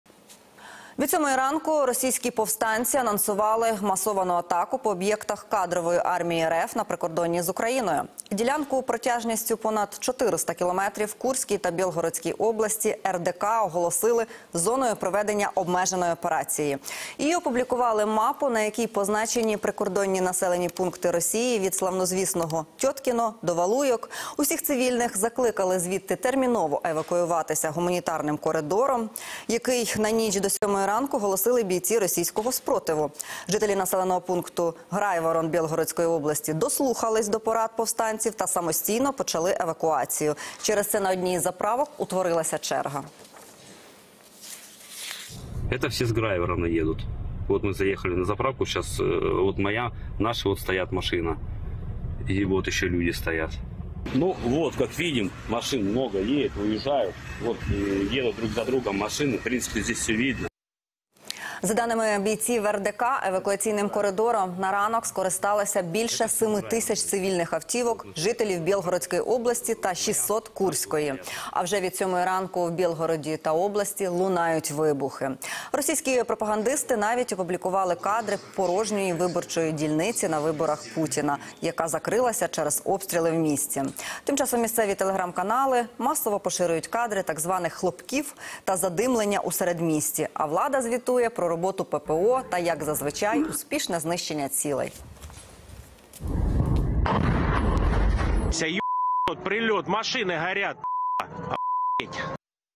Як просувається операція по звільненню росії від путіна?
Легіон "Свобода Росії", Російський добровольчий корпус (РДК) і Сибірський батальйон анонсували масовану атаку по "путінським військовим" у Курській і Бєлгородській областях, у зв'язку з чим відкрили гуманітарний коридор для цивільних і російських військовослужбовців із 21:00 сьогоднішнього дня до 7:00 15 березня. Деталі в сюжеті!